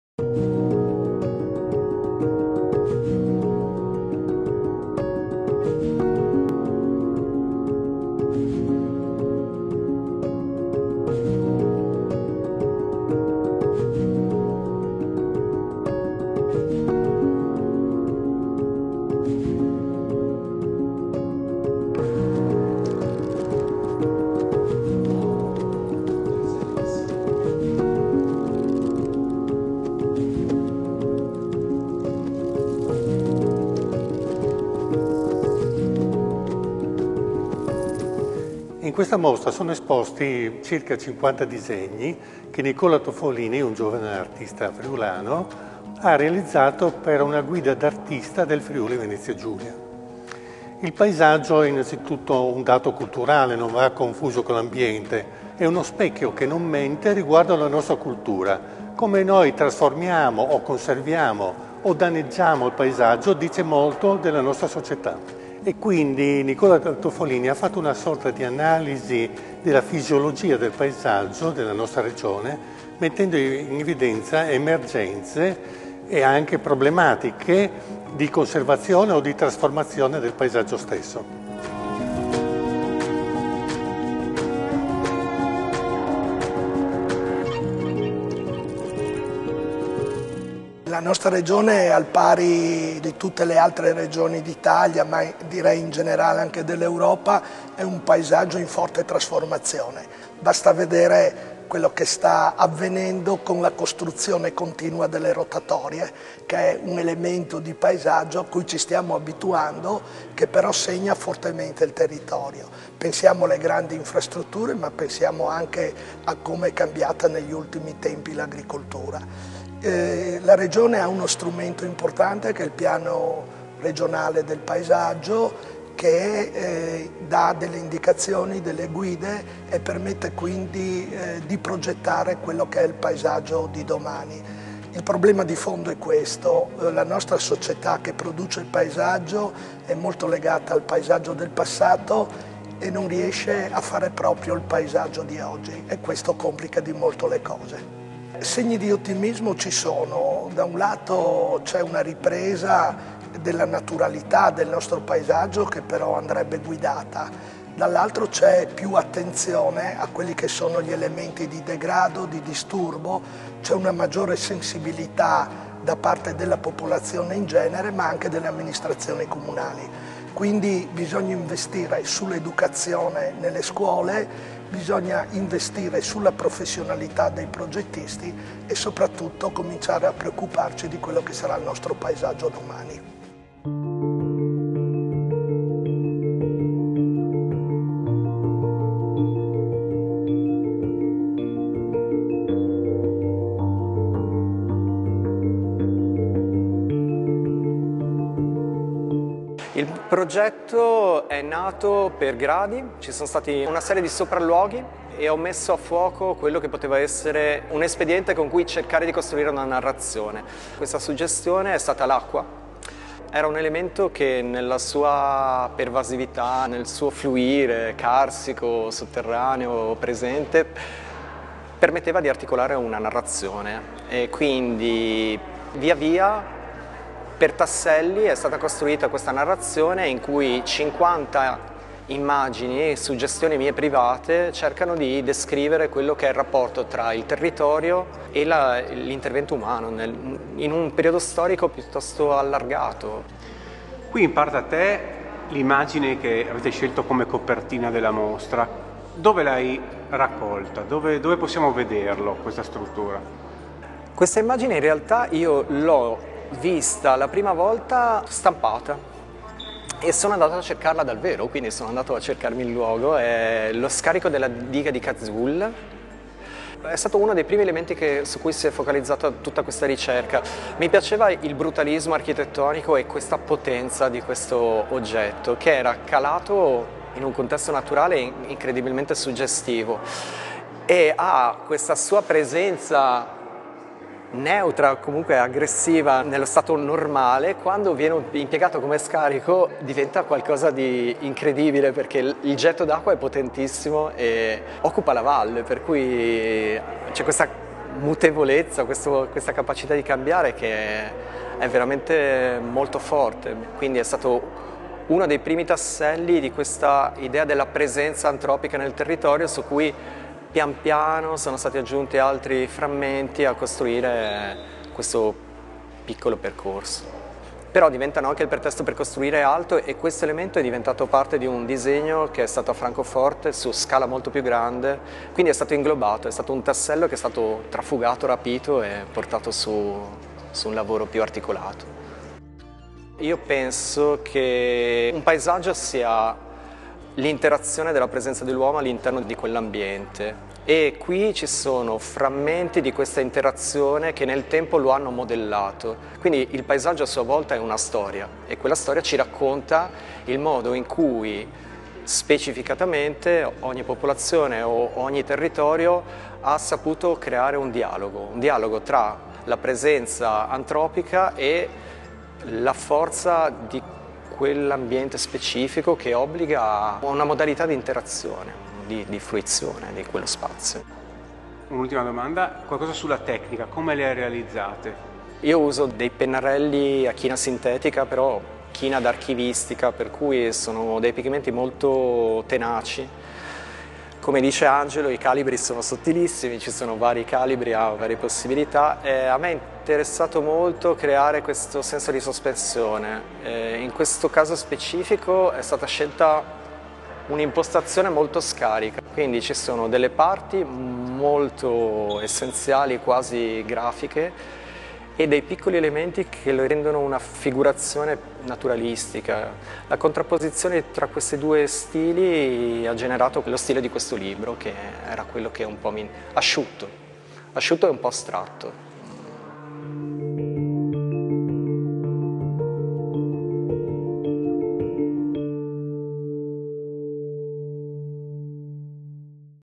[CICP] Angelo Bertani, Mauro Pascolini e Nicola Toffolini: videointerviste
Pordenone 26 marzo - 29 maggio 2022
IN MOSTRA ALLA GALLERIA SAGITTARIA PORDENONE I PAESAGGI DEL FRIULI VENEZIA GIULIA AI TEMPI DELL’ANTROPOCENE: PER LA PRIMA VOLTA IN ESPOSIZIONE I DISEGNI DELL’ARTISTA UDINESE NICOLA TOFFOLINI, ATTIVO FRA IL FRIULI E FIRENZE, AUTORE DELLE TAVOLE DELLA GUIDA D’ARTISTA DEDICATA AL FRIULI VENEZIA GIULIA, PUBBLICATA DALLE EDIZIONI CENTRO DI.